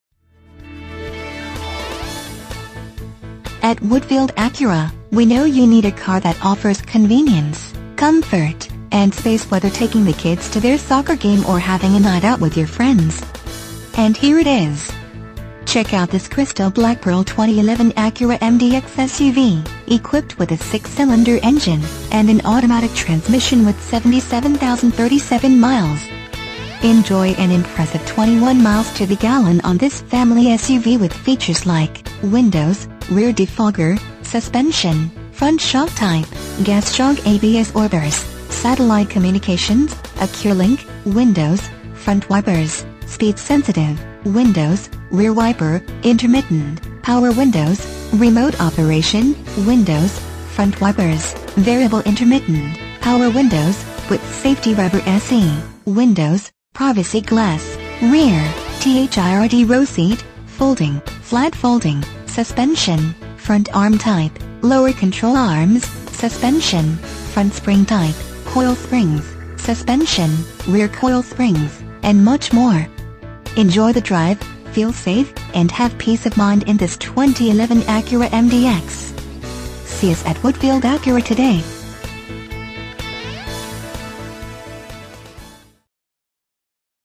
2011 Acura MDX Woodfield Acura Hoffman Estates, IL 60195
Acura MDX At Woodfield Acura, we know you are looking for a vehicle to solve everyday task. Whether loading the kids soccer equipment or having a night out with your friends. You will have the room you need making those everyday task an ease to accomplish. You will love this Crystal Black Pearl Acura Mdx SUV, equipped with a 6 Cylinder engine and an automatic transmission with 77,037 miles. Enjoy an impressive 21 miles to the gallon on this family SUV with features like Windows, Rear Defogger, Suspension, Front Shock Type: Gas Shock Absorbers, Satellite Communications, Acuralink, Windows, Front Wipers: Speed Sensitive, Windows, Rear Wiper: Intermittent, Power Windows: Remote Operation, Windows, Front Wipers: Variable Intermittent, Power Windows: With Safety Reverse, Windows, Privacy Glass: Rear, Third Row Seat, Folding: Flat Folding, Suspension, Front Arm Type: Lower Control Arms, Suspension, Front Spring Type: Coil Springs, Suspension, Rear Coil Springs, Real Time Traffic, Suspension, Rear Gas Shock Absorbers, Suspension, Stabilizer Bars: Front And Rear, Third Row Seat, Type: 50-50 Split Bench, Sport Suspension, Exterior Mirrors, Heated, sunroof, Anti-Trapping Feature, and much more. Enjoy the drive, feel safe, and have peace of mind in this Acura Mdx. See us at Woodfield Acura today!
MILES: 77,037
VIN:2HNYD2H63BH519483

Woodfield Acura
1099 w. Higgins Rd.